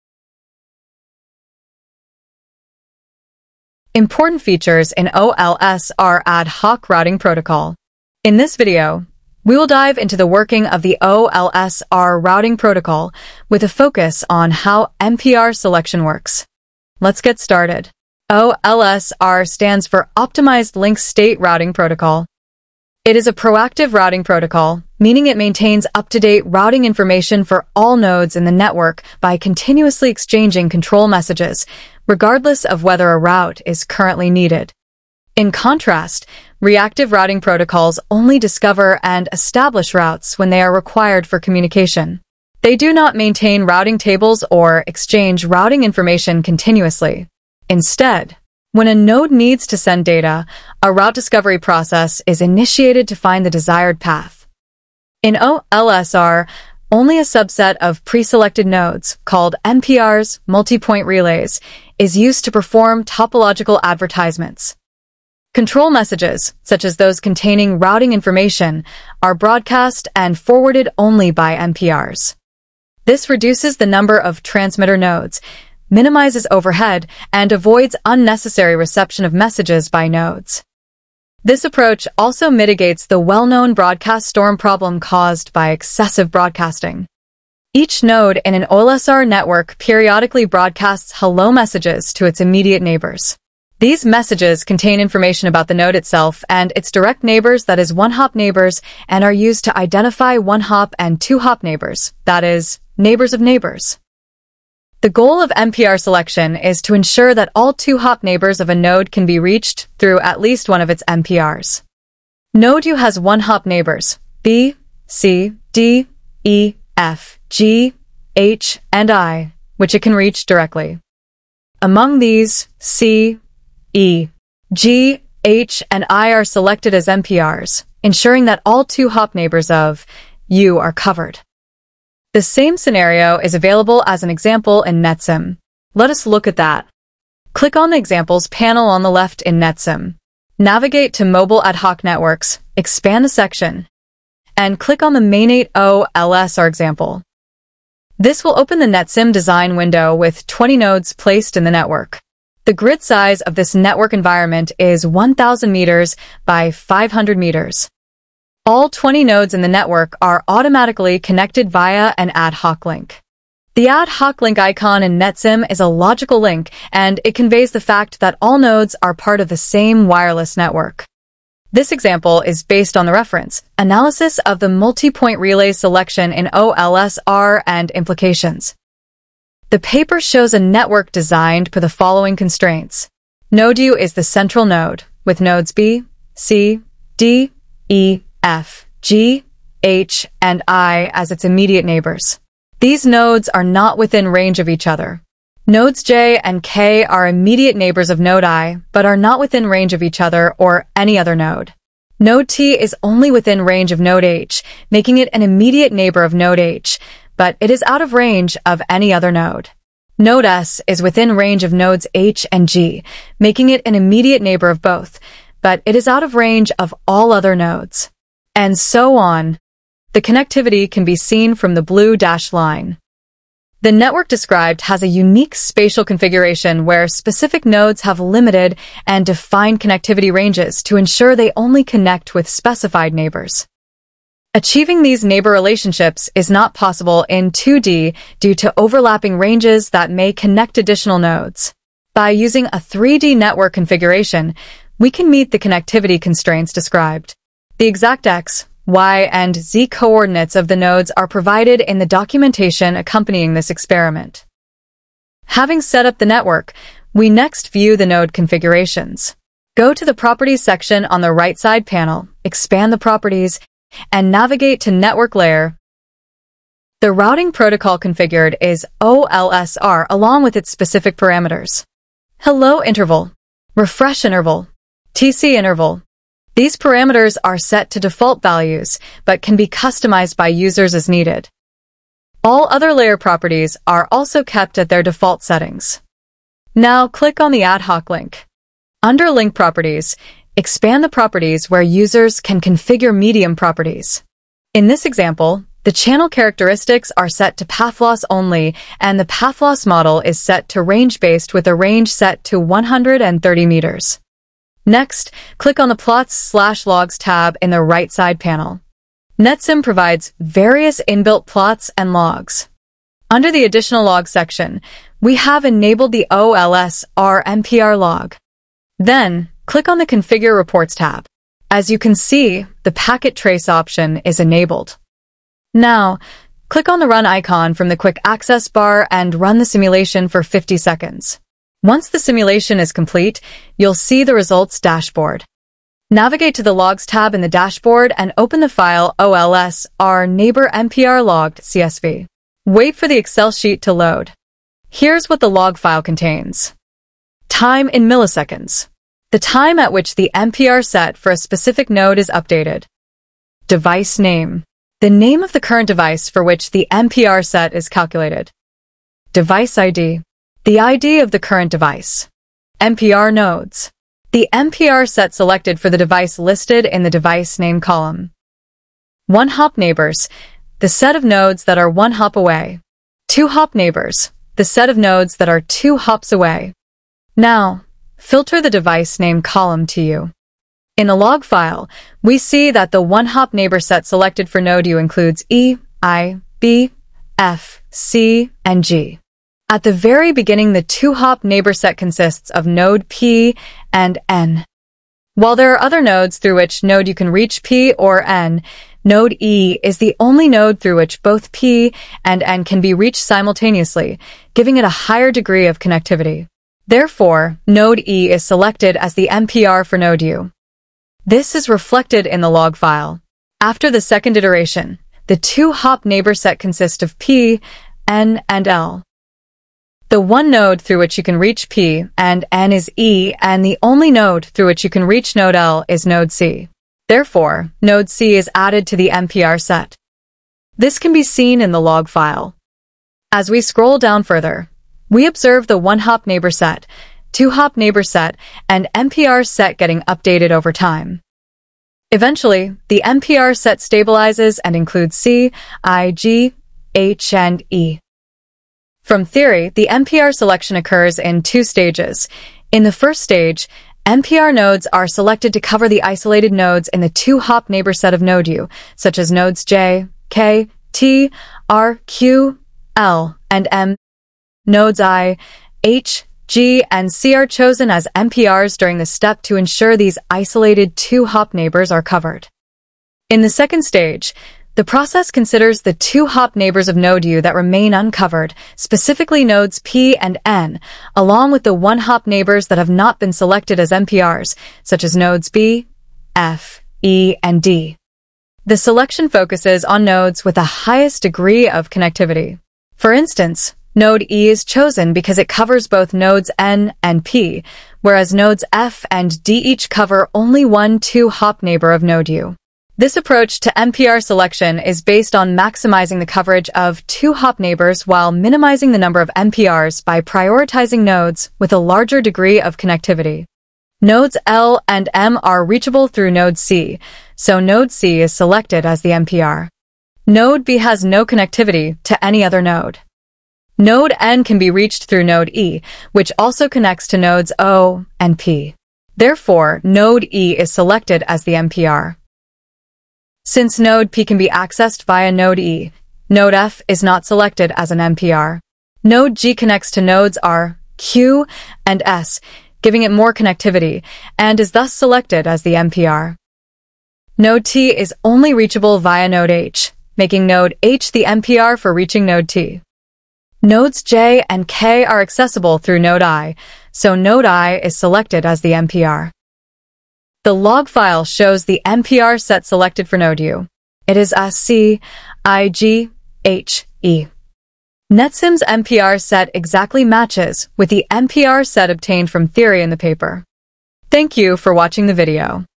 OLSR Protocol in Mobile Ad hoc Networks (MANETs) | NetSim
In this video, we explore the Optimized Link State Routing (OLSR) protocol in Mobile Ad Hoc Networks (MANETs). We explain how OLSR operates and the Multi-Point Relay (MPR) selection algorithm. With NetSim, we demonstrate the MPR selection process and compare the output against a well known research paper.

Topics Covered:
Introduction to OLSR in MANETs
Understanding MPRs in OLSR
The MPR selection algorithm
Practical demonstration using NetSim